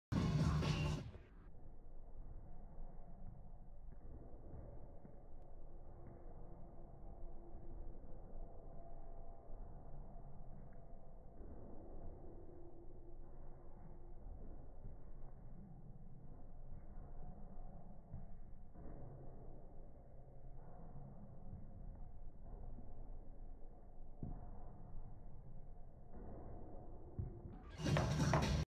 VICTORIA ARLEN DWTS - Agility Ladder Training
SEE VICTORIA'S LEGS IN FULL SPEED ON DANCING WITH THE STARS, MONDAY NIGHTS EVERY WEEK!

Victoria Arlen Training at her family's Gym (Project Walk Boston) pre DWTS. This Lateral 2in agility ladder movement took her around just over 2 weeks to become proficient in!

Amongst other types of training, we had been working on Forward 2ins for the 2 weeks before this and Running Gait training in general for four+ months & Walking Gait training for endless months before that.